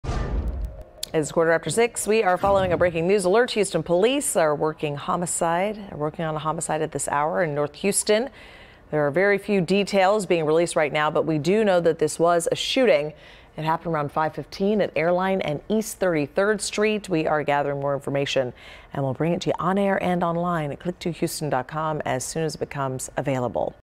Police investigate fatal shooting in north Houston
Police are investigating a homicide Tuesday near Houston’s Northline neighborhood.